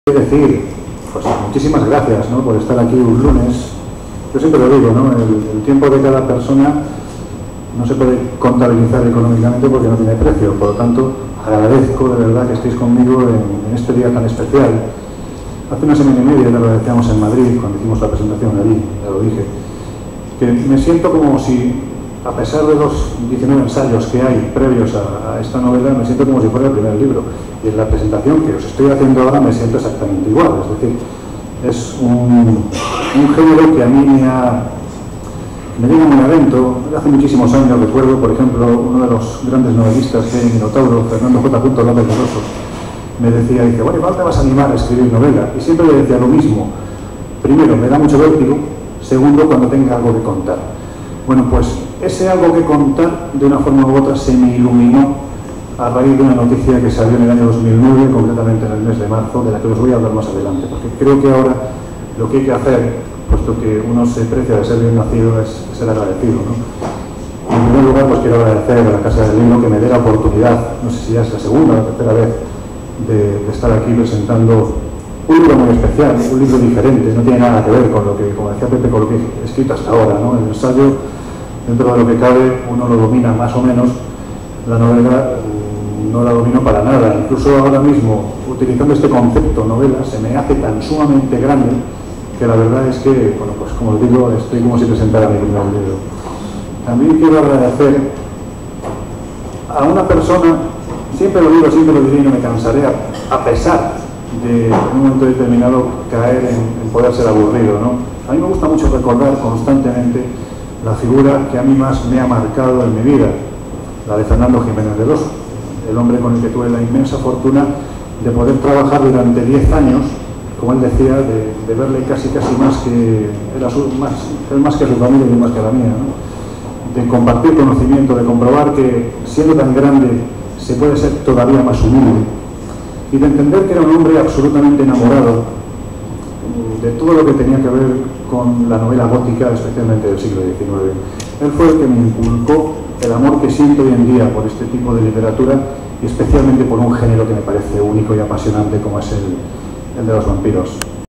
El Vampiro De Silesia
Lorenzo Fernández Bueno,ensayista y actual miembro de la conocida tertulia"Zona Cero", dentro del programa "La Rosa De Los Vientos" de Onda Cero. También ha presentado el espacio"Rastreadores de Misterios"en Telemadrid, director de la revista ENIGMAS del Hombre y del Universo; presenta su primera novela "El Vampiro De Silesia", en la Casa del Libro de Barcelona. 25 de Noviembre 2013